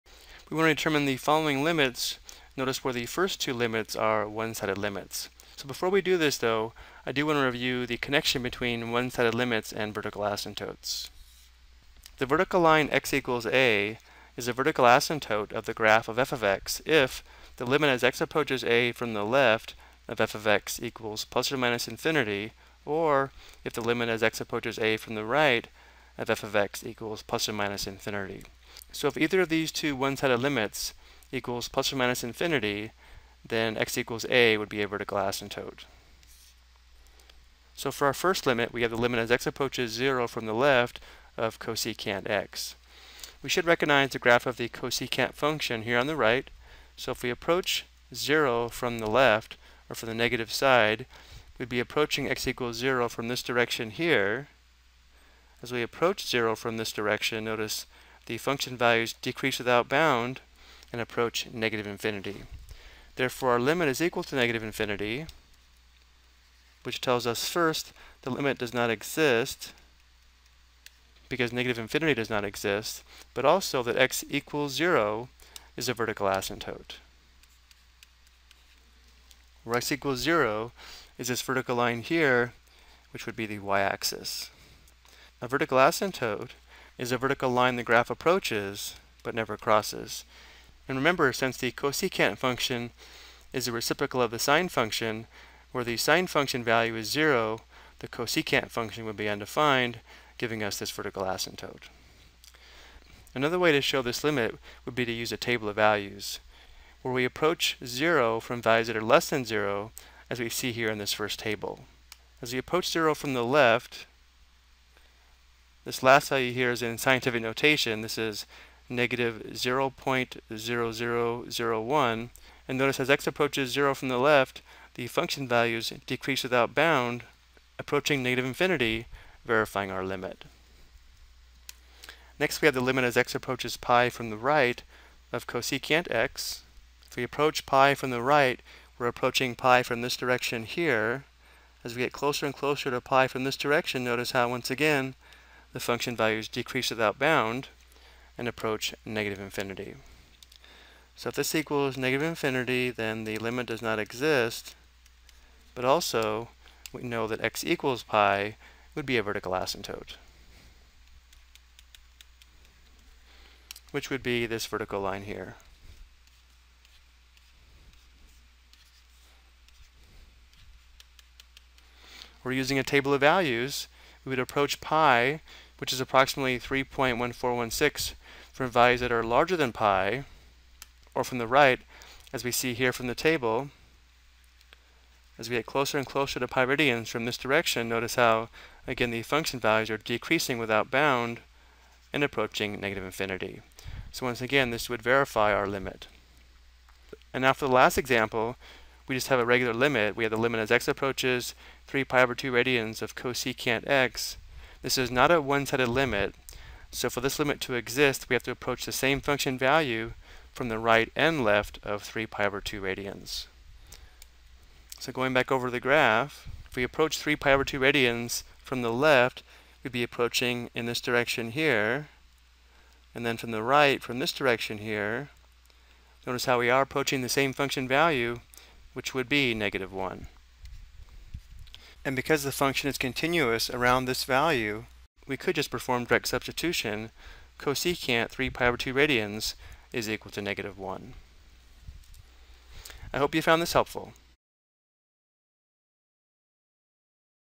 Ex 5: One-Sided Limits and Vertical Asymptotes (Cosecant Function)
This video explains the connection between one-sided limits and vertical asymptotes.